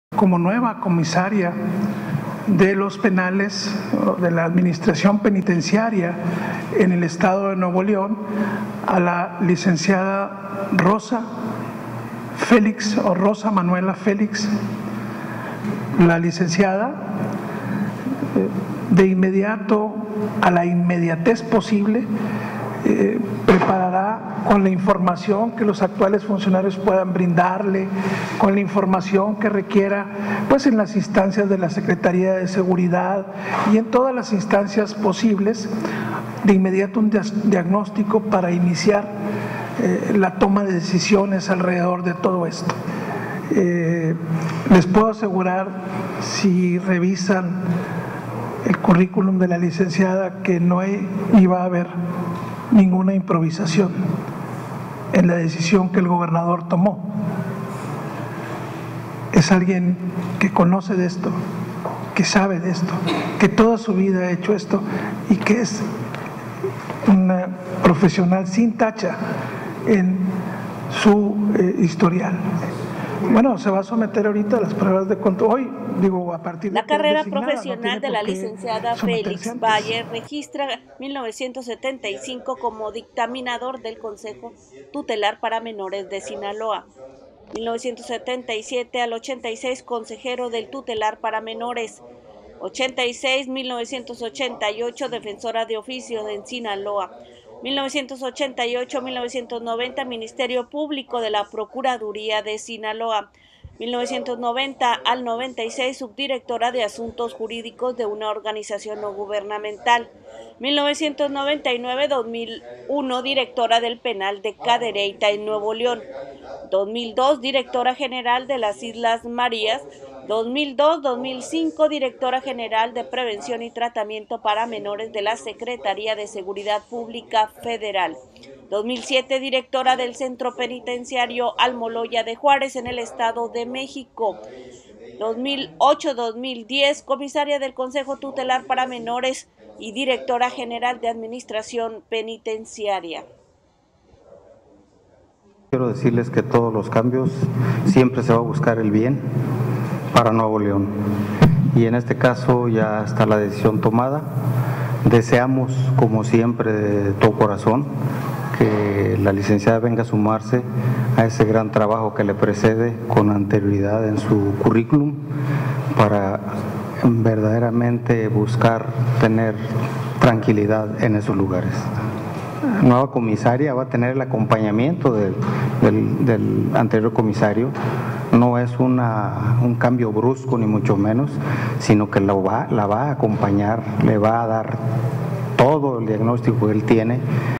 Nueva Comisaria
El Gobierno del Estado nombró a la abogada Rosa Manuela Félix Valles como Comisaria del sistema penitenciario de Nuevo León  quien ha ocupado cargos en cárceles como Almoloya de Juárez en el Estado de México y las Islas Marías.